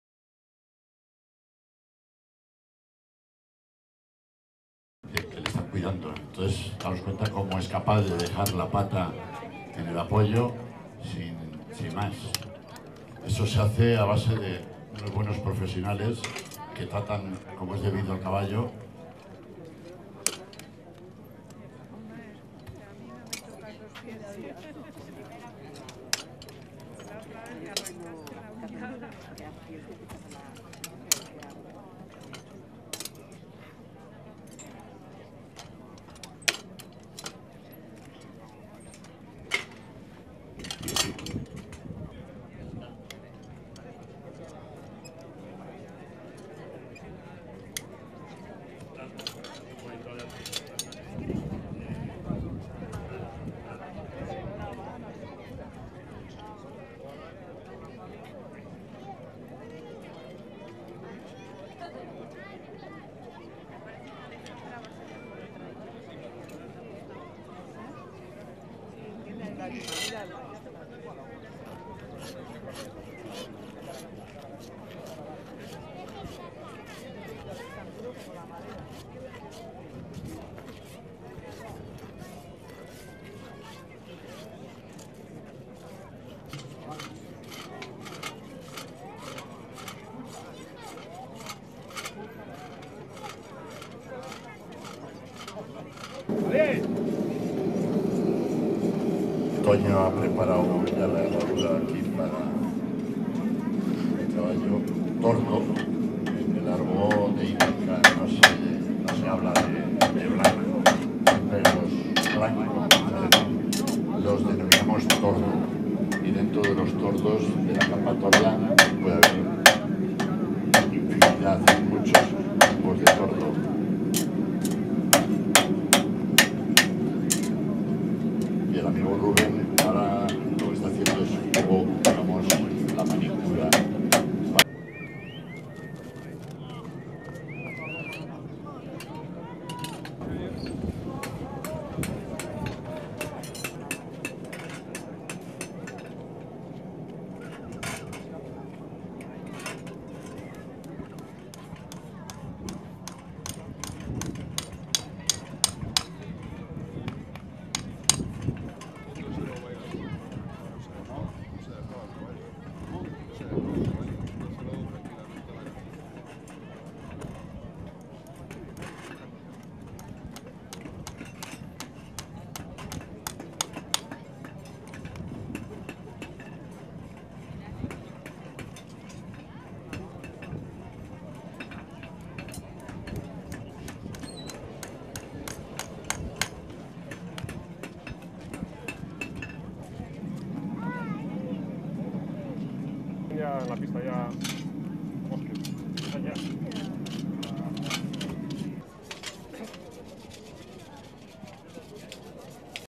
Herraje de caballo
Imágenes de la forja y herraje de un caballo, donde podemos comprobar como el maestro herrero, retira uno por uno los remaches de la vieja herradura para después limpiar la zona del casco con diferentes herramientas. Posteriormente, una vez calentada la herradura y habiéndole dado la forma adecuada, procede a su colocación.